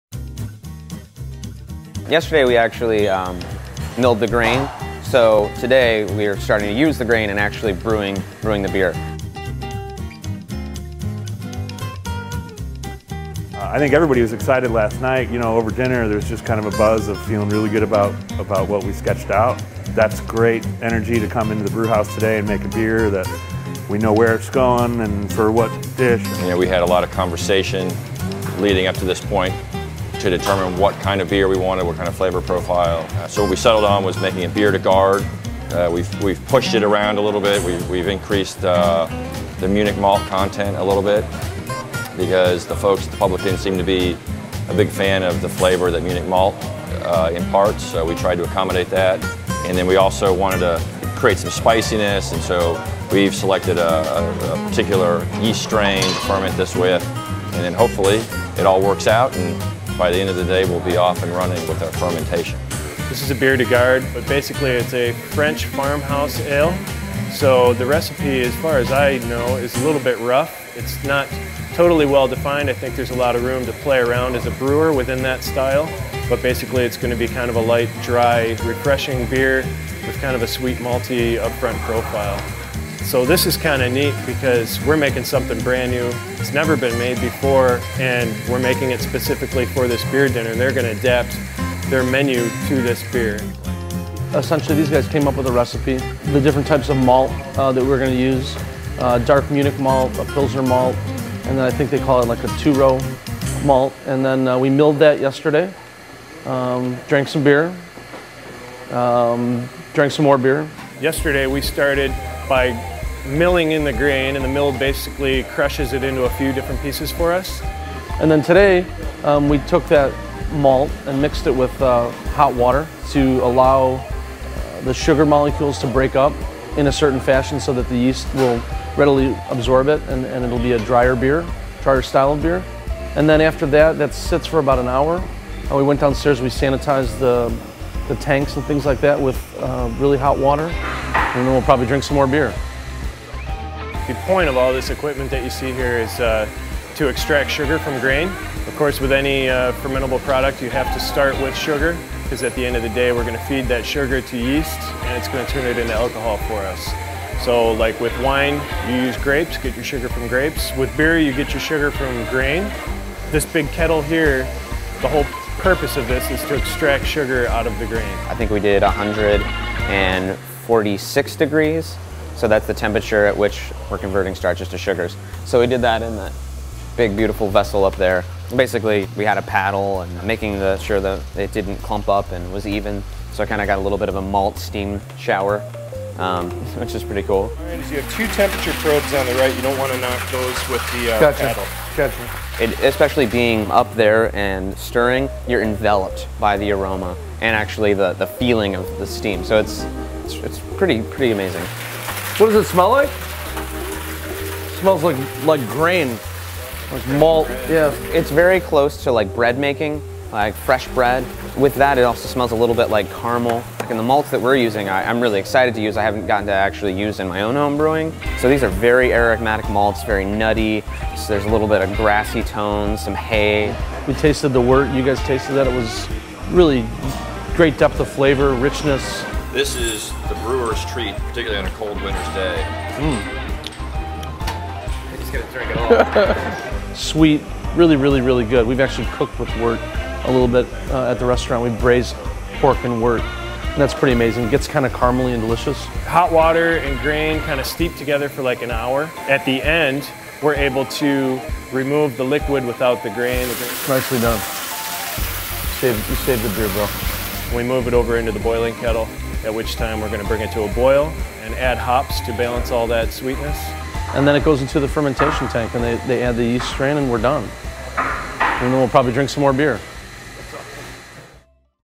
Paul Kahan Makes More Beer
As the Executive Chef for restaurants like Blackbird, The Publican, Avec, and the Velvet Hour, Paul Kahan has very little time to mess about. When an opportunity to hand-craft a beer for an upcoming beer dinner at The Publican presented itself, Paul and his team jumped at the chance to work with the New Holland Brewing Company.

In this second episode of three, Paul makes beer.